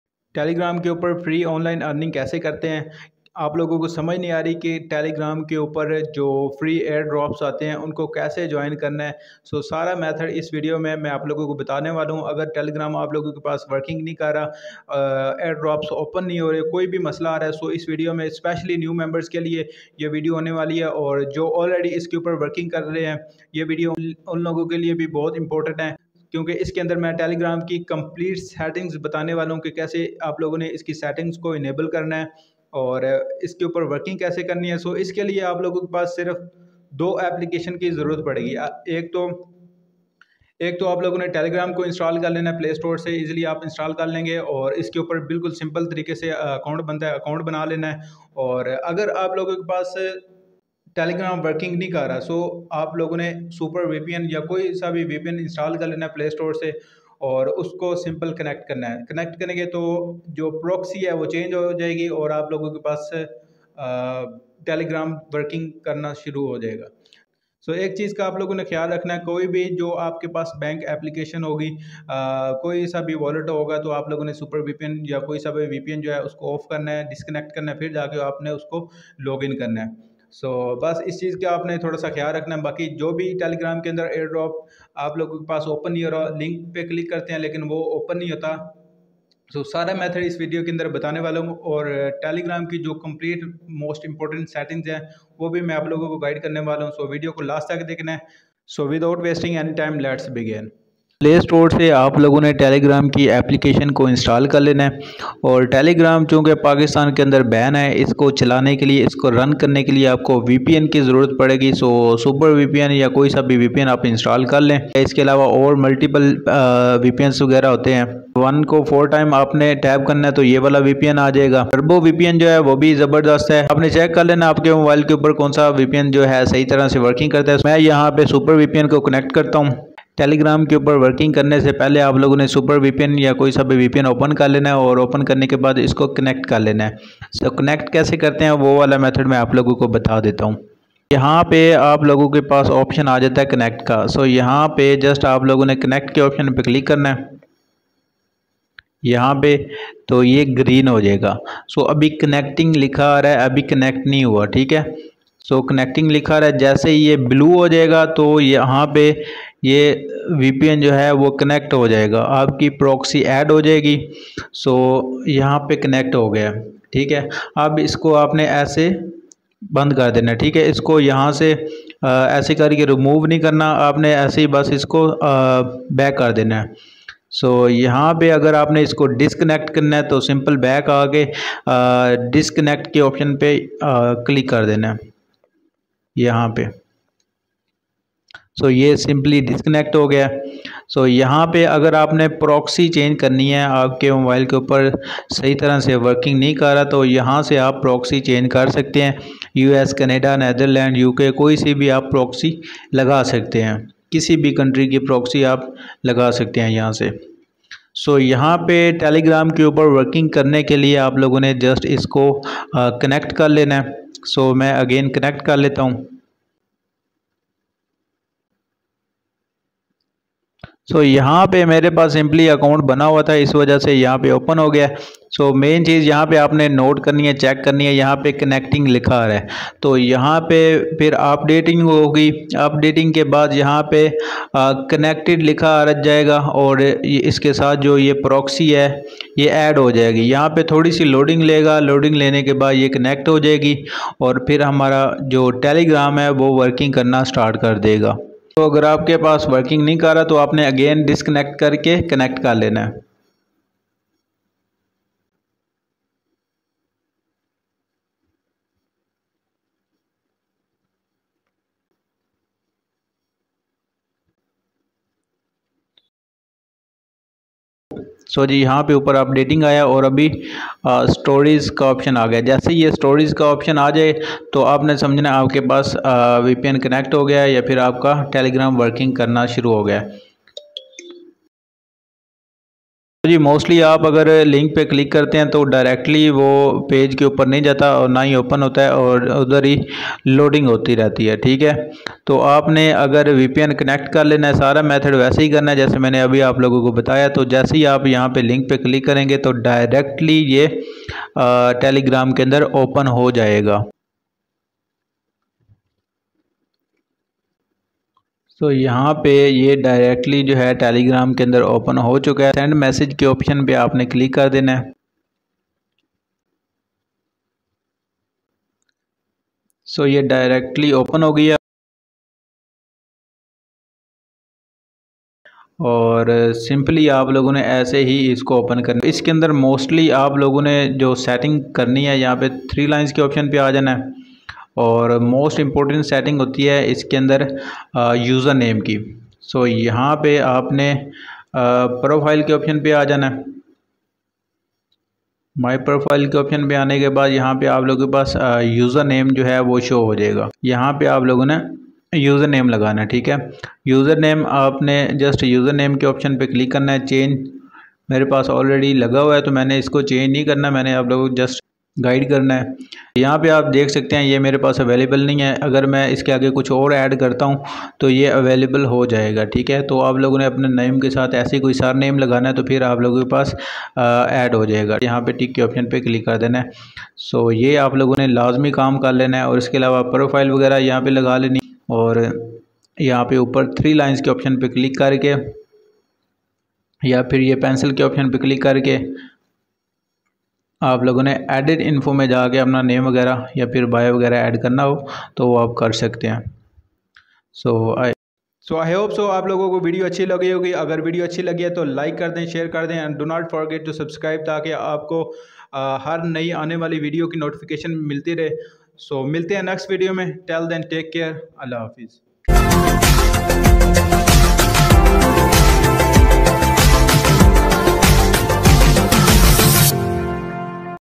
Free Telegram Earning | Login Connecting & Settings Method Solve || Telegram Sy Kasy Pasy Kamayn
Earn Rs900 Daily From Telegram | How to Make Money by using Telegram  - Shoaib Akram

in this video I will explain how to earn money from Telegram channel۔ Telegram is like a whatsapp but here we can earn money Using Telegram channel